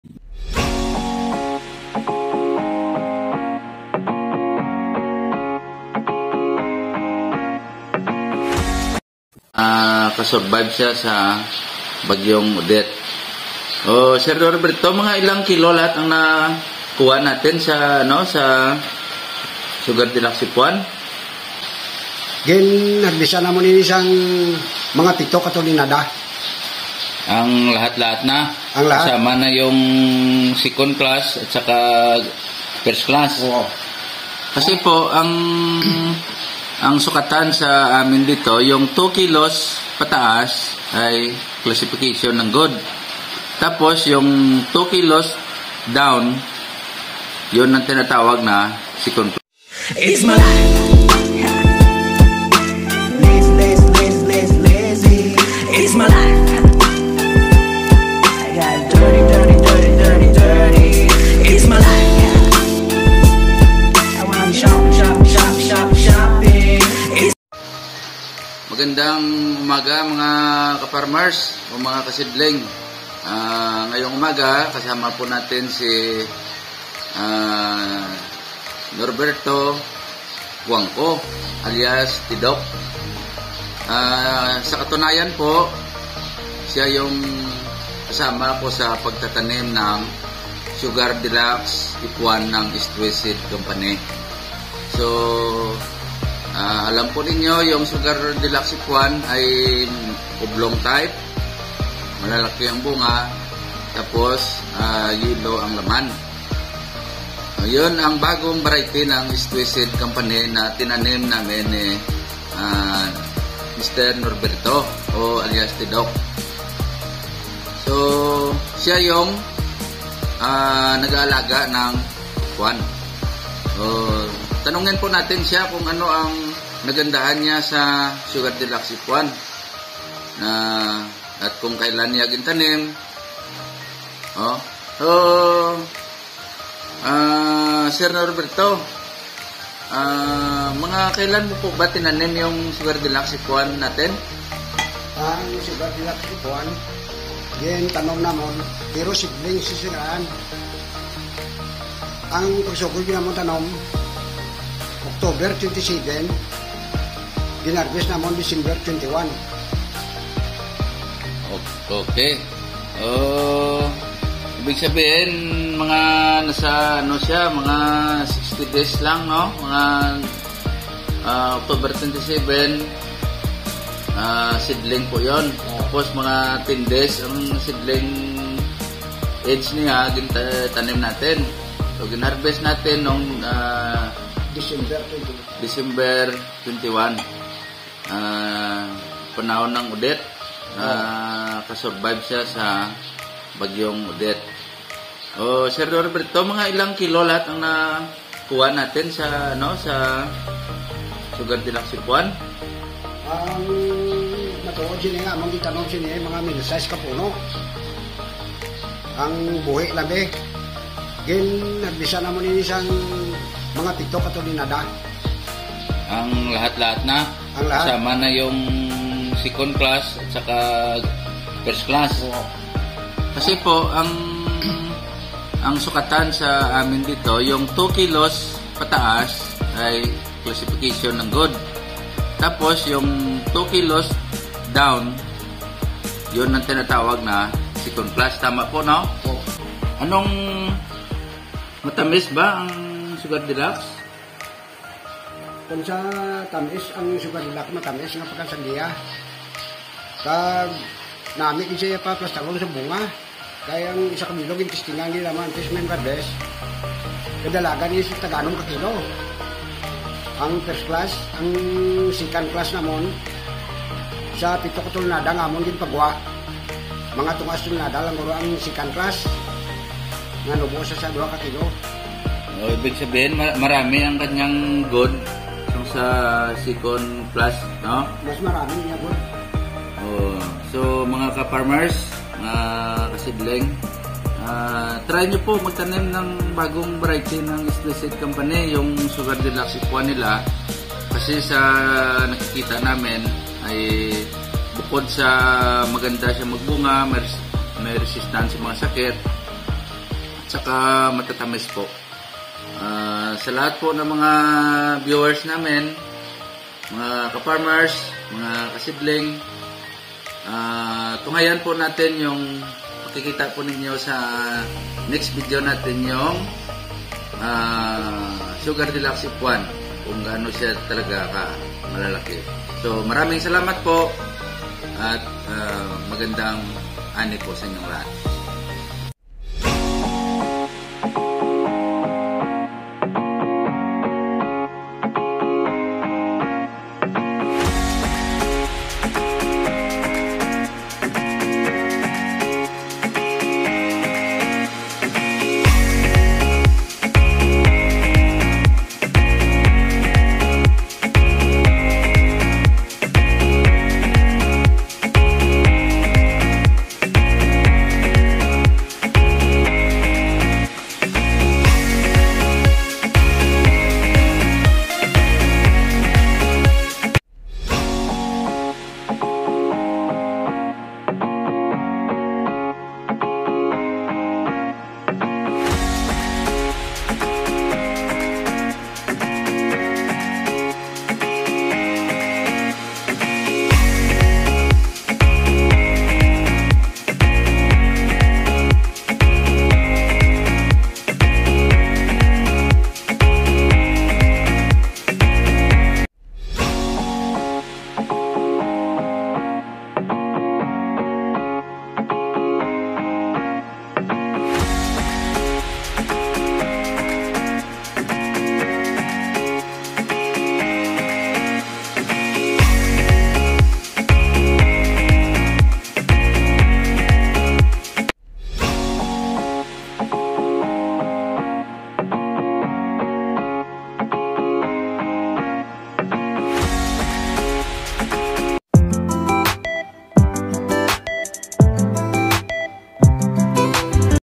How to grow watermelon in Negros by Ruben & Norberto! find out?harvesting
show us how Norberto and Ruben, share their experiences on how to be a successful farmer in planting watermelon.